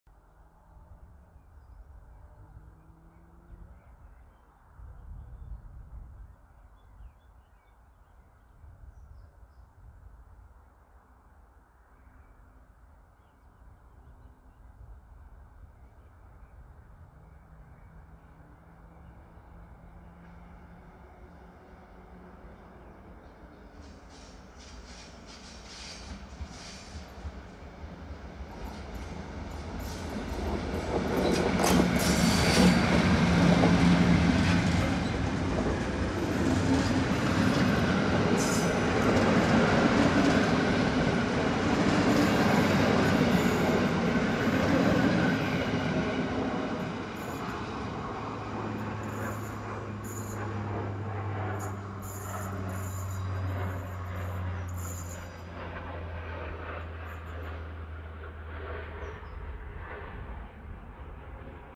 EP09-004 | IC 30116 Galicja [4K]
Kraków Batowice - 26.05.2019
Świeżutka i pachnąca naprawą P4 EP09-004 ze składem IC 30116 Galicja do Przemyśla